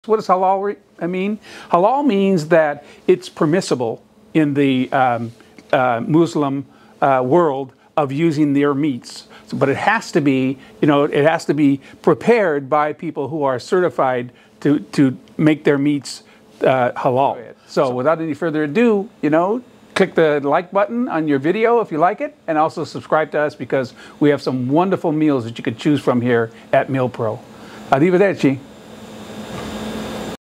What is Halal Food?
In This Video Chef Dan Catanio Goes Over what Halal Food is.